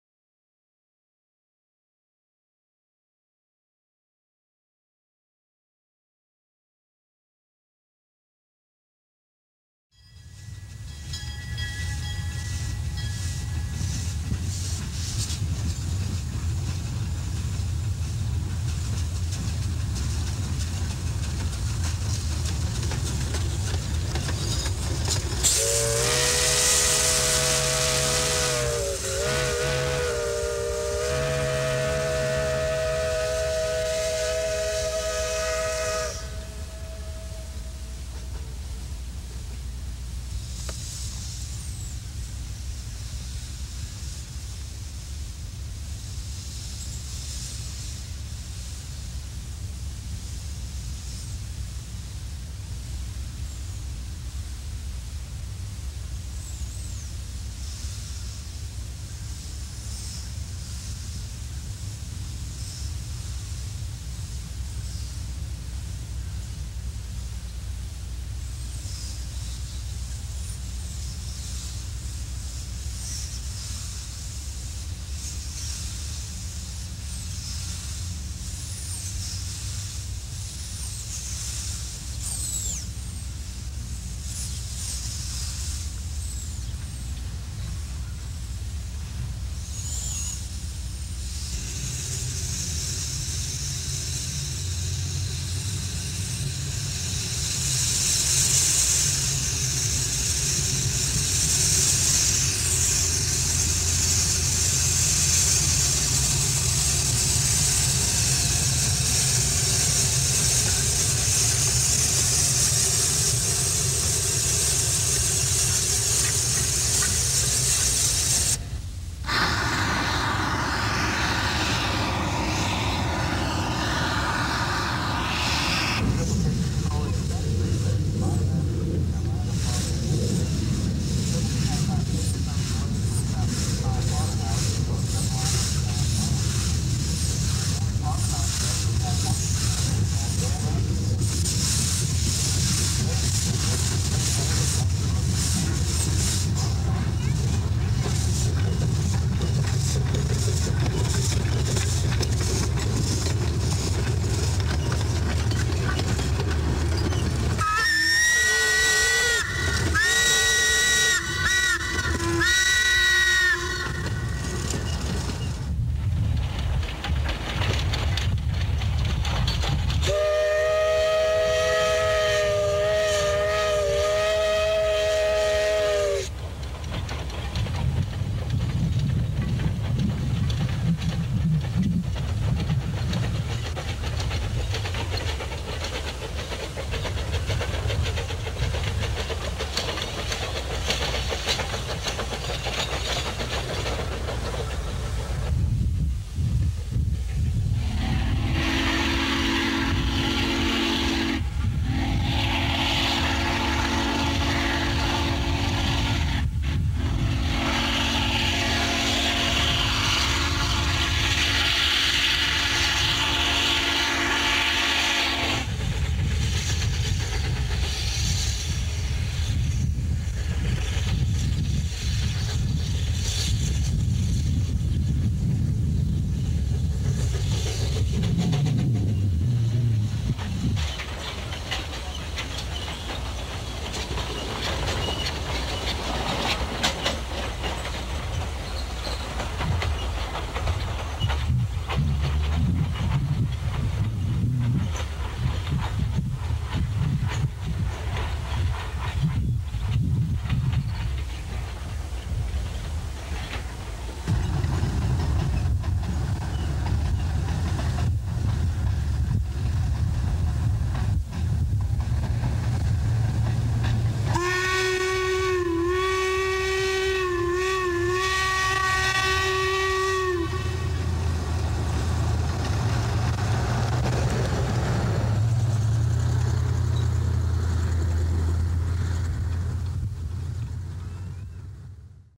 Cripple Creek & Victor - Colorado's 2-Foot Gauge - 1981
Colorado's Cripple Creek & Victor Narrow Gauge Railroad is seen in the summer of 1981 running between Cripple Creek and Anaconda.  The locomotives in use this day are No. 2, a 1936 German-built 0-4-0 (converted from an 0-4-0T) and No. 3, an 0-4-0T built by Porter in 1927 for a mine in Mexico.  The railroad continues in operation today, more-or-less unchanged from the way you see it here.